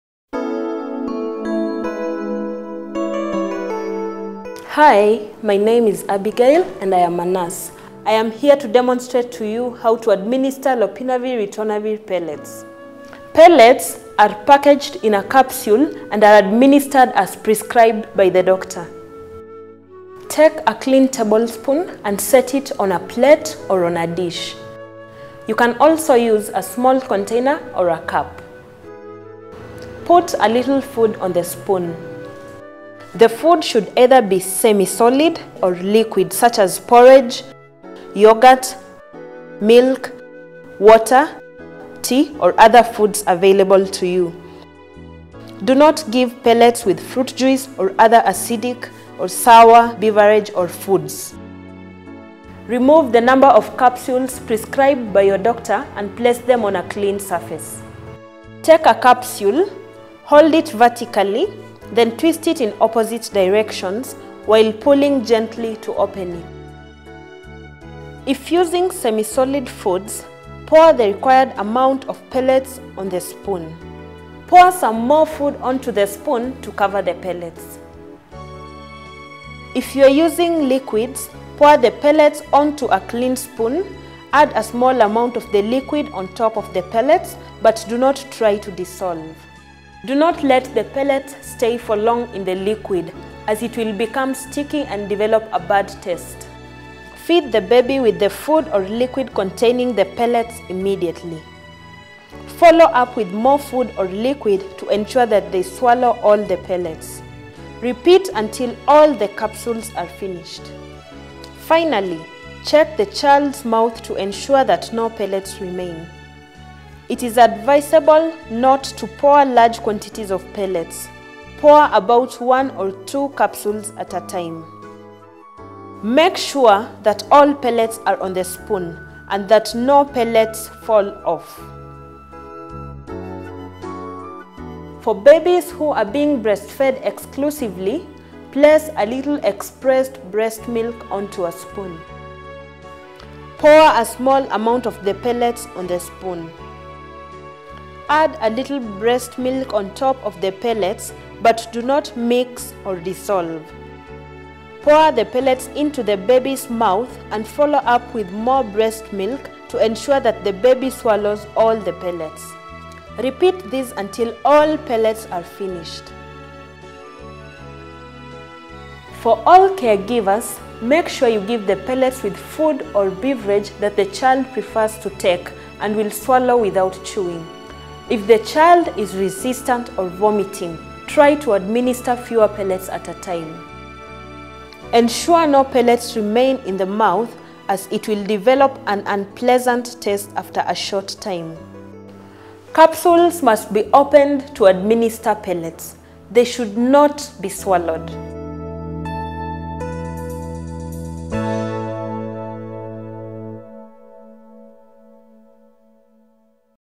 Instructional Video: How to Administer Lopinavir/Ritonavir Pellets to HIV-Infected Children
This is an instructional video that offers direction to caregivers on how to administer Lopinavir/Ritonavir pellets to HIV infected children. The pellets are a new formulation of an existing syrup that has a bitter taste, needs refrigeration and contains over 40% alcohol. This pellet formulation are taste masked and can easily be taken with food. This video was developed following lessons from the LIVING Study; an implementation Study which is trying to understand the acceptability and effectiveness of the pellets currently taking place in selected African countries.